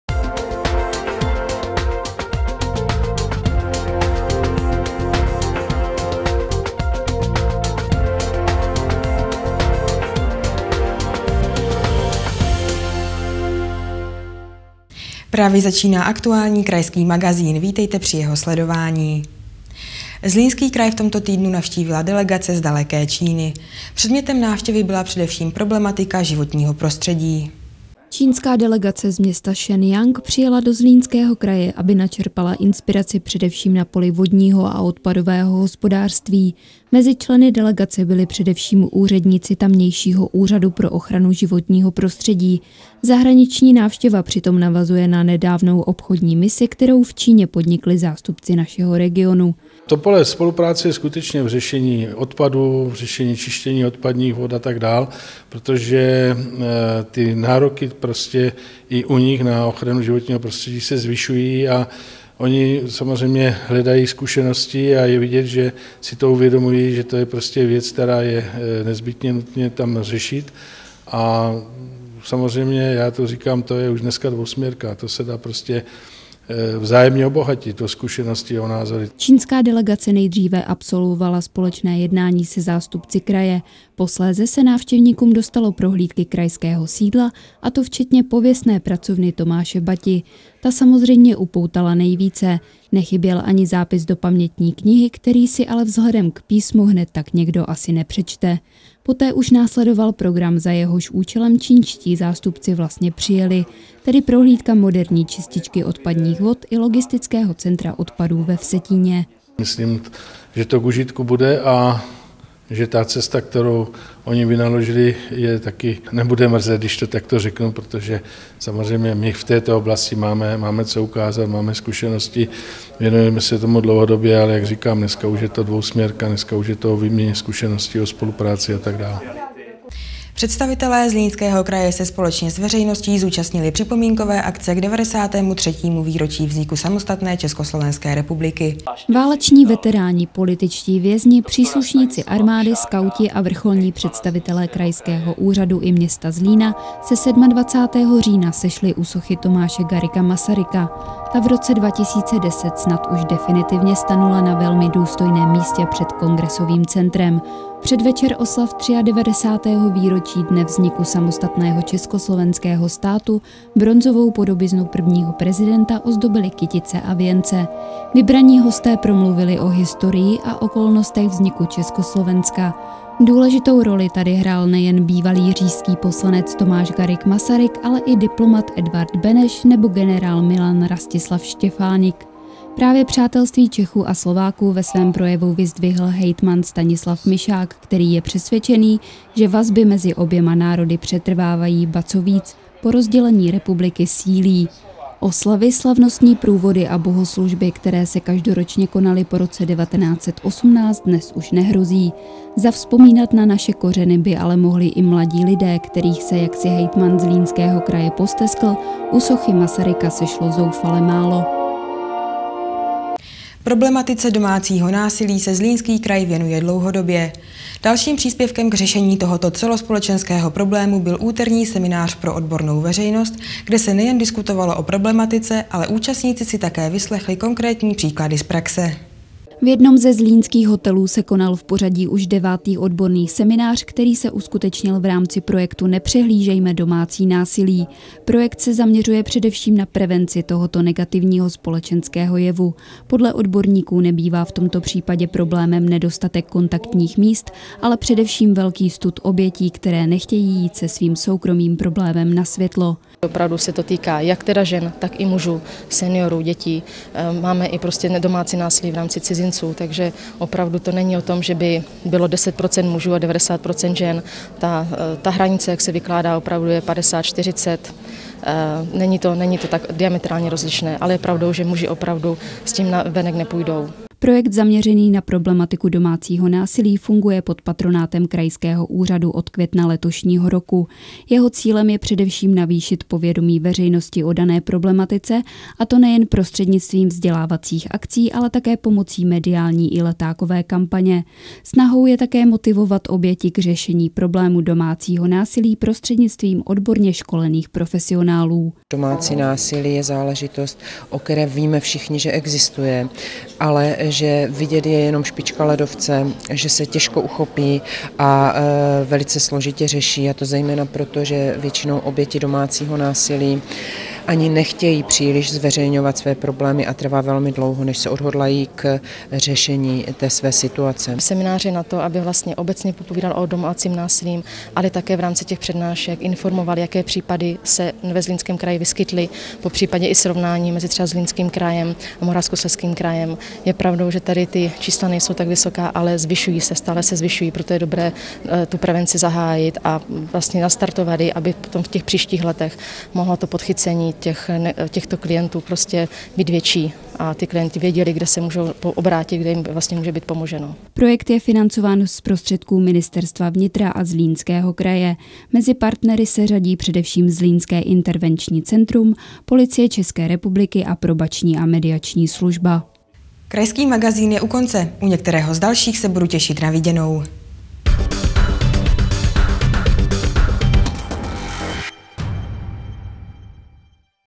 Kraj 28.10.2011
Zlínský kraj navštívila delegace z Číny / Představitelé Zlínského kraje se zúčastnili přípomínkové akce k založení Československé republiky / Ve Zlíně se uskutečnil seminář na téma domácího násilí